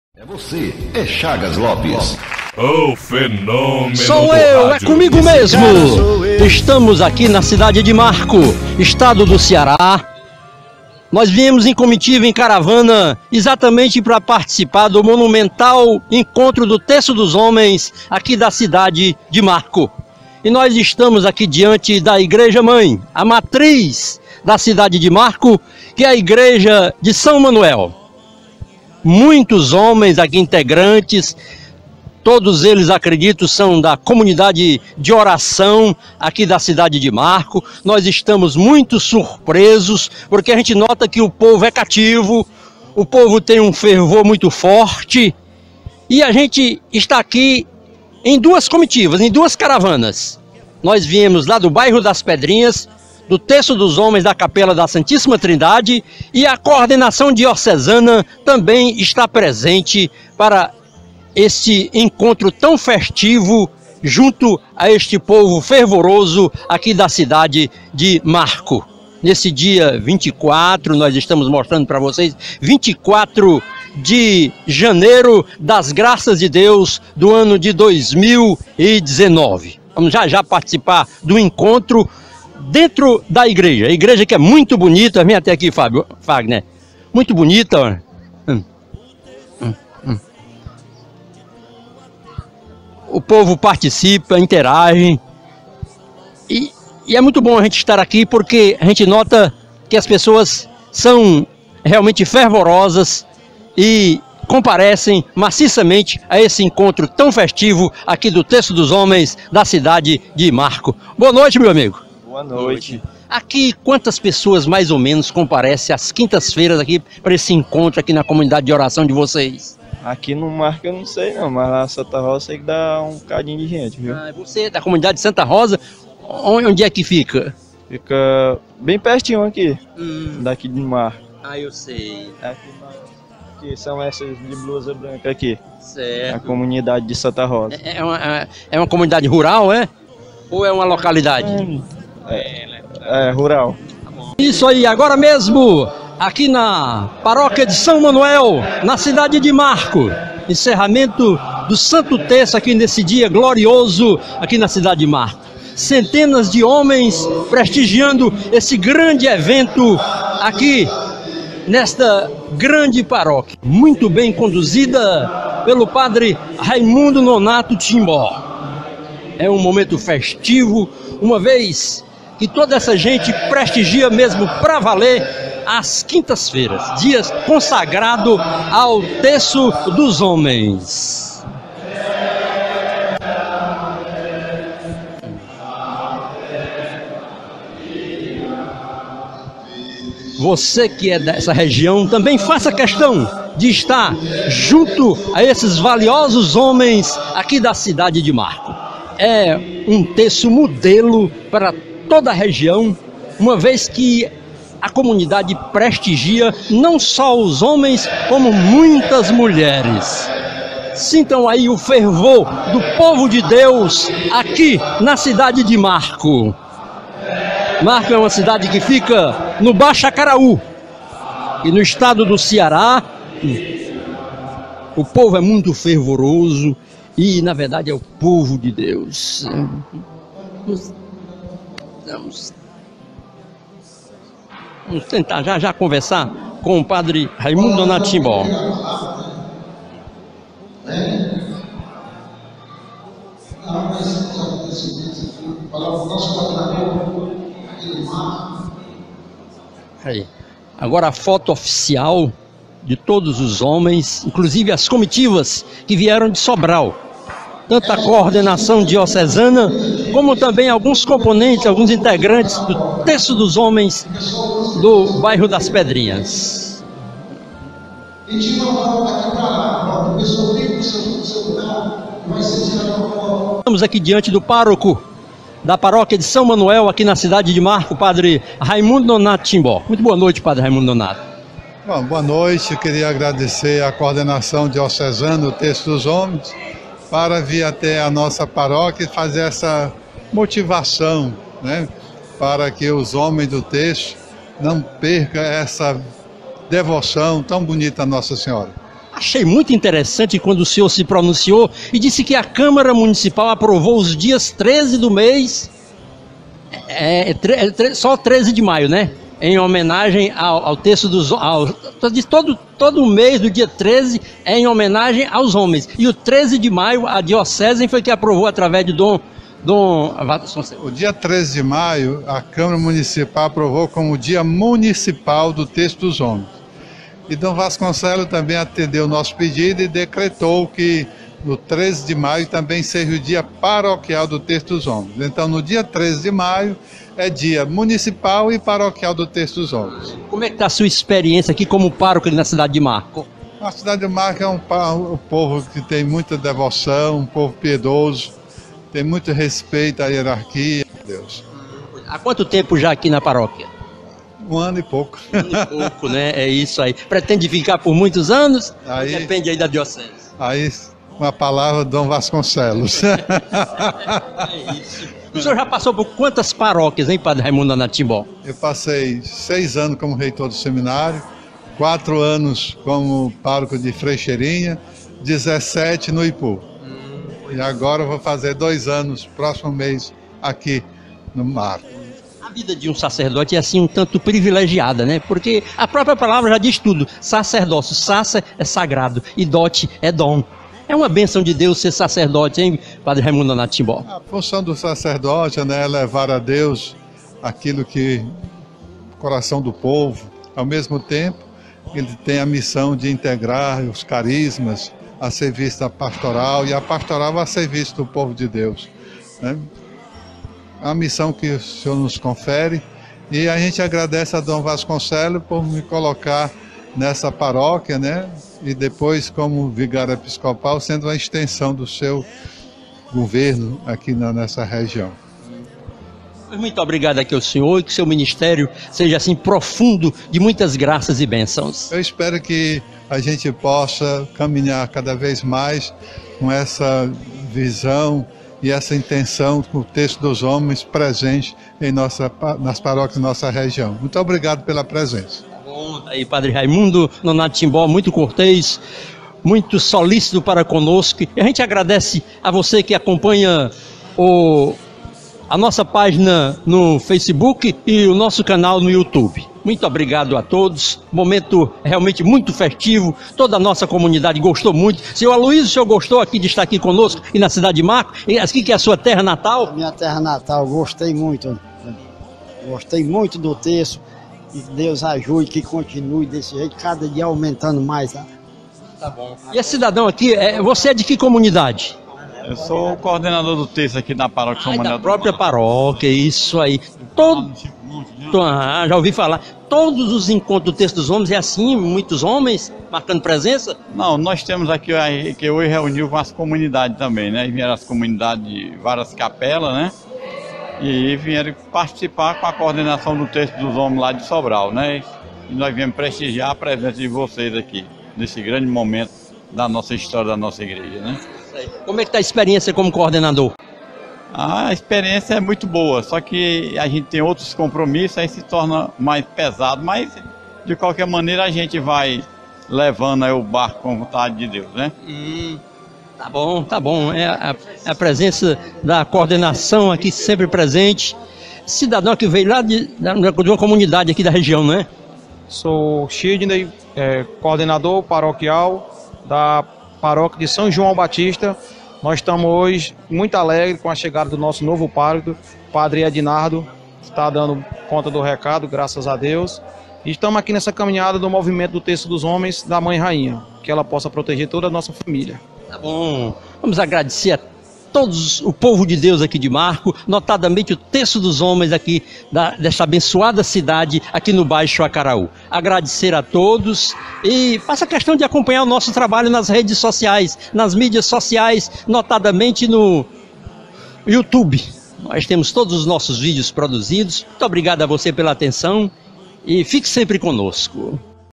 Radialista Chagas Lopes na Paróquia de Marco-Ce
Paróquia de São Manoel, cidade de Marco_Ce, conversa com o Pároco Raimundo Nonato Timbó⛪